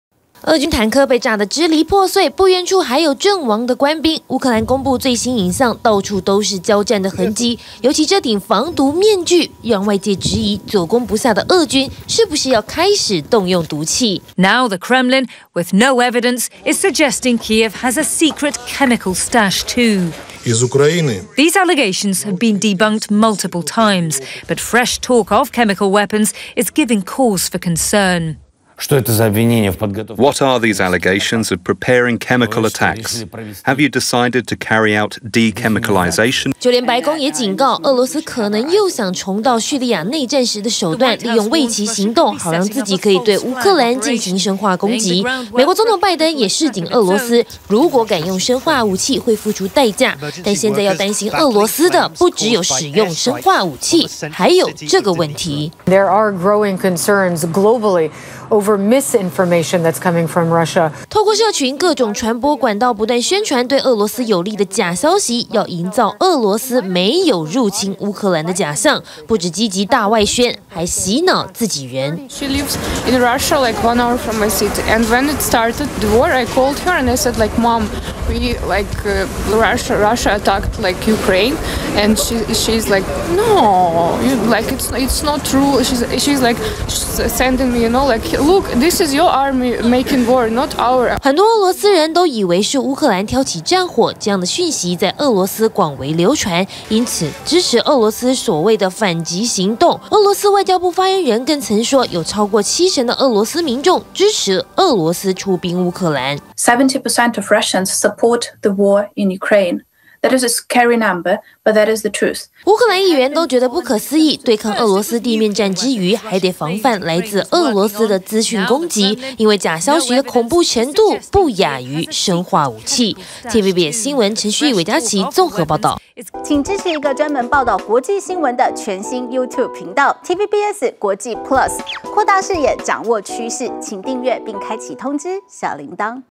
交戰驚見防毒面具 俄羅斯恐發動「生化戰」｜TVBS新聞
俄羅斯侵略烏克蘭超過兩個星期，卻久攻不下，烏克蘭公布的最新畫面中，竟然發現俄軍殘留的防毒面具，烏克蘭總統澤倫斯基也懷疑，是不是生化攻擊的前奏？白宮也警告，俄羅斯可能利用偽旗行動，對烏克蘭進行生化攻擊。